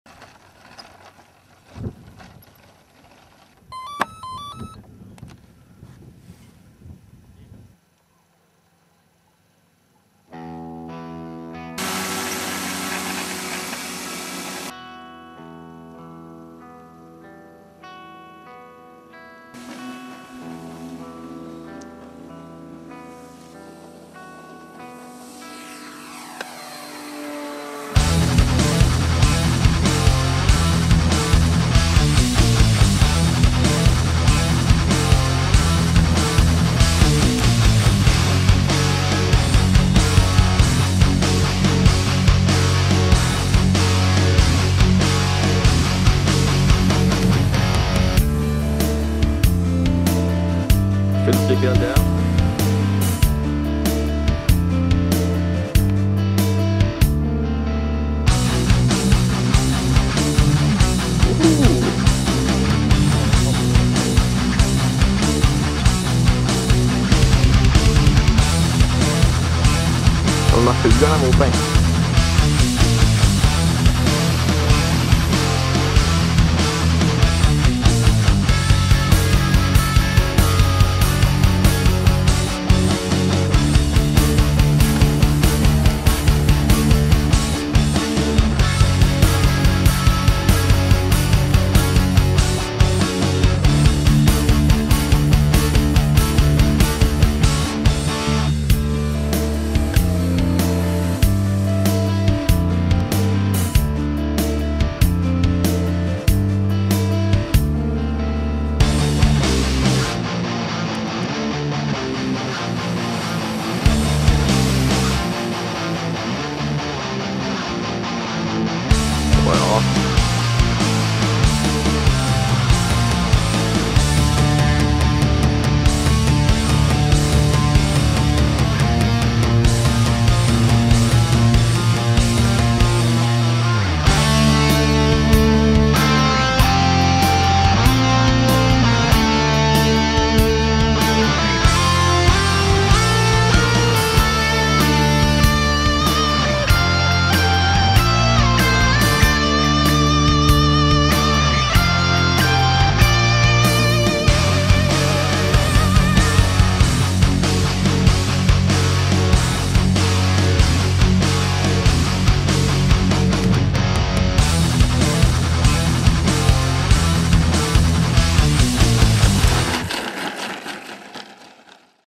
Mosquito debut
Toralf flies his new Mossie